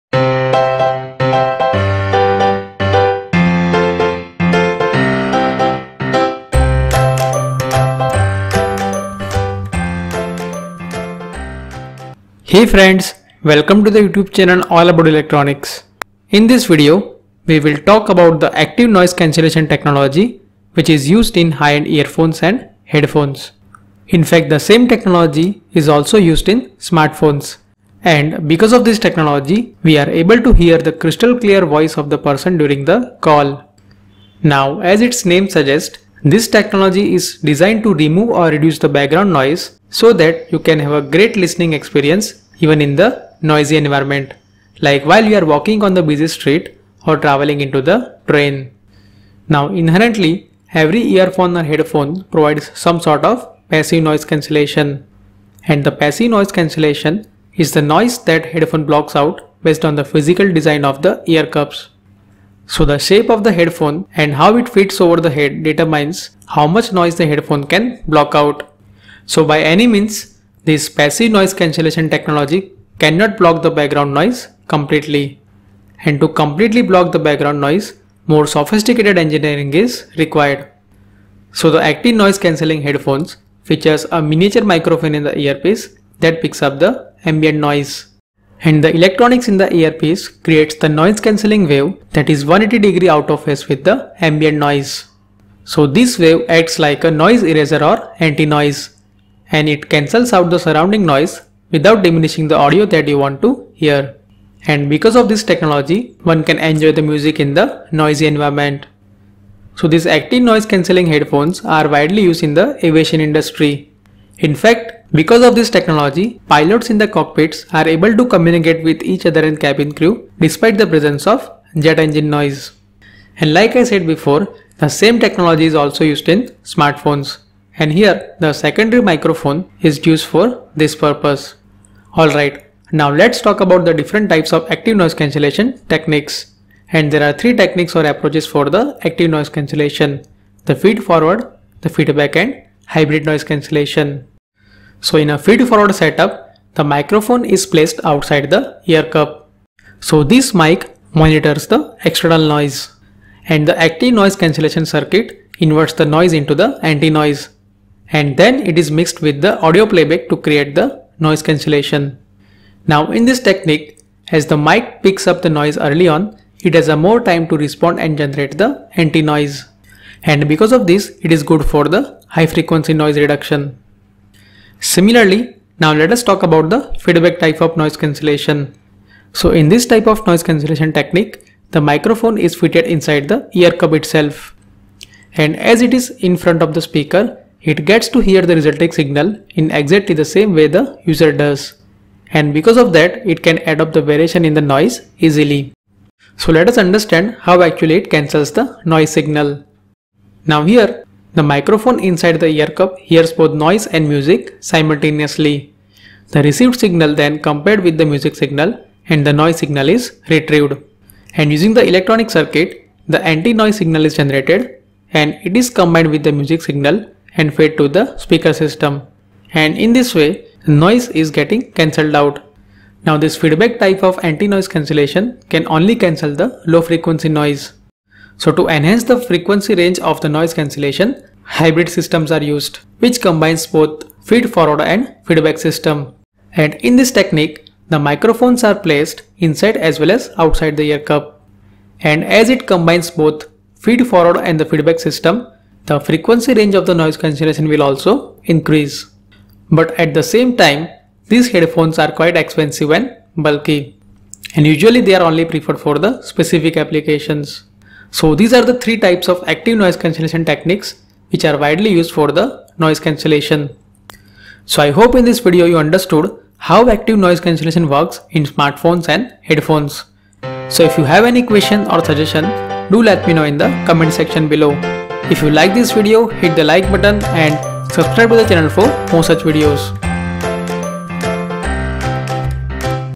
Active Noise Cancellation Explained
In this video, the active cancellation technology which is used in smartphones and high-end headphones has been explained.

The following topics have been covered in the video:

0:34 What is Active Noise Cancellation and how it works?

2:13 Different types of Active Noise Cancellation (Feed-Forward, Feedback and Hybrid ANC)

What is Active Noise Cancellation (ANC)?
In the active noise cancelation, the miniature microphone near the ear cups detects the background noise and using the electronic circuit (controller) 180 degree inverted signal (Anti-Noise Signal) is generated.
This Anti-Noise signal is mixed with the (Music + Noise ) signal, which effectively cancels out the noise signal and the listener can enjoy the noise-free music.
As these ANC headphones are battery operated and involve the high-end engineering design, they are bulky as well as quite expensive (compared to  the passive headphones. )

 Three types of ANC techniques are widely used for the noise suppression.
1) Feed-Forward  ANC
2) Feedback  ANC
3) Hybrid ANC

Feed-forward ANC
In a feedforward setup, the microphone is placed outside the ear cup. The mic hears the noise before the person.
The mic monitors external noise, the Active Noise Cancellation circuit inverts it into “anti-noise,” then mixes it with audio playback to create a noise cancellation.
As the mic picks up the noise early on,  it has more time to respond and generate the anti-noise. And it is good for high-frequency noise reduction.
But, during the generation of the anti-noise signal, the path traveled by the actual noise signal also needs to be considered. If the actual noise is coming from some weird angle then the path traveled by the noise will be slightly different and it will reach to the user's eardrum little delayed.
And anti-noise will not be able to cancel it completely. In some cases, if the path difference is more then instead of canceling it, it will actually amplify the noise.

Feedback ANC
In this technique,  as the mic is fitted inside the ear cup and in front of the speaker, it gets to hear the resulting signal in exactly the same way the user does. And because of that, it can adopt the variation in the noise easily.
Because the round-trip time, which includes monitoring, comparing, and creating anti-noise, is longer than that for feed-forward anti-noise, feedback type ANC is only effective for canceling low-frequency noise.

Hybrid ANC

It combines both feed-forward and feedback system. And in this technique, the microphones are placed insides as well as outside the ear cup. And the user should receive the best noise cancellation. Also using this method, the frequency range for the noise cancellation can be enhanced. The Hybrid ANC headphones are the most expensive among the different types of ANC.

This video will be helpful to everyone in understanding how active noise cancellation works in smartphones and headphones.